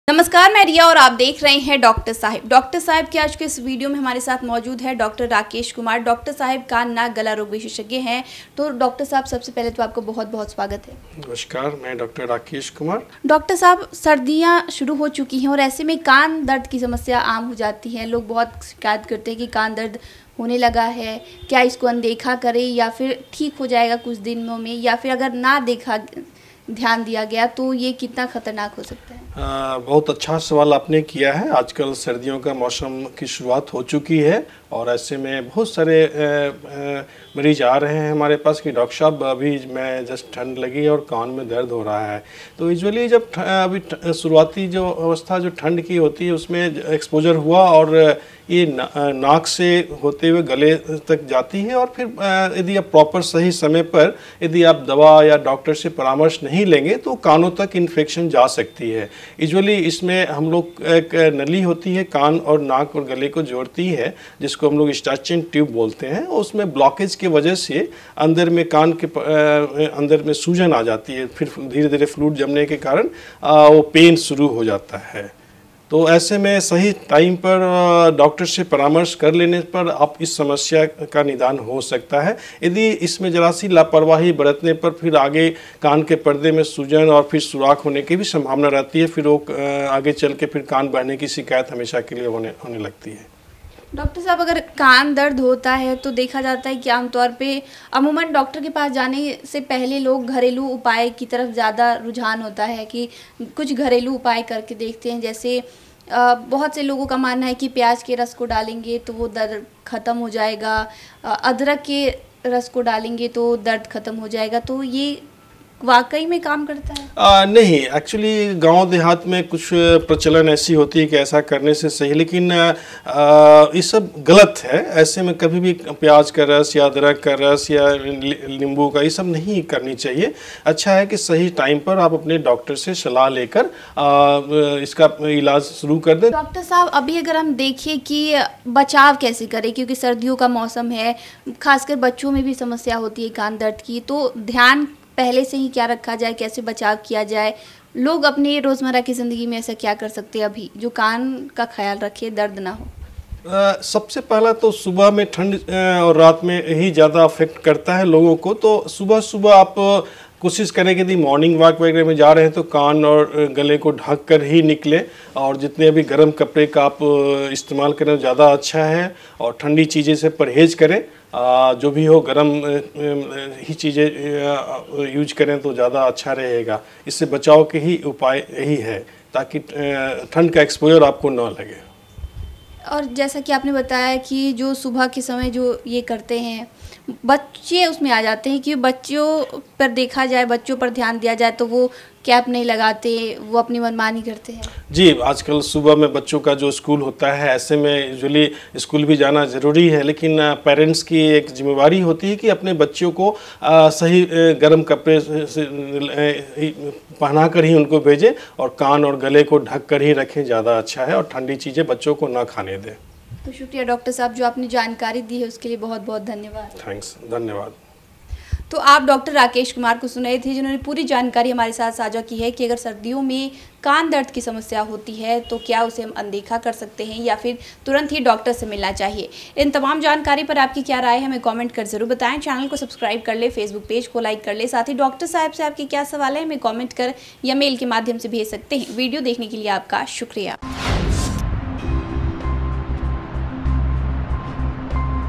Dr. Rakesh Kumar से जानें, Winter में Ear Pain के बढ़ने का कारण क्या है, इलाज कैसे होगा ?
देखें वीडियो, बिहार के जाने-माने नाक, कान, गला रोग विशेषज्ञ डॉ. राकेश कुमार बता रहे, सर्दियों में आपका भी दर्द करता है कान, कारण कहीं ये तो नहीं...